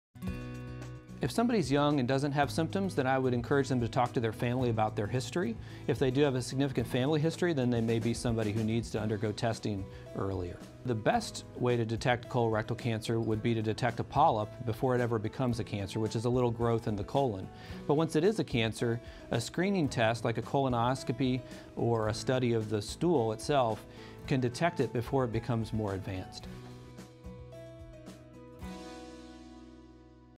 How is colorectal cancer detected in the early stages? | OSUCCC – James
In this video, colon and rectal surgeon Alan Harzman, MD, recommends that young people with no symptoms of colorectal cancer learn about their family history. Those who do have a significant family history of the disease may need to be screened earlier. The best way to detect a polyp, which is a small growth in the colon, is through a colonoscopy screening, which can detect polyps before they become cancer.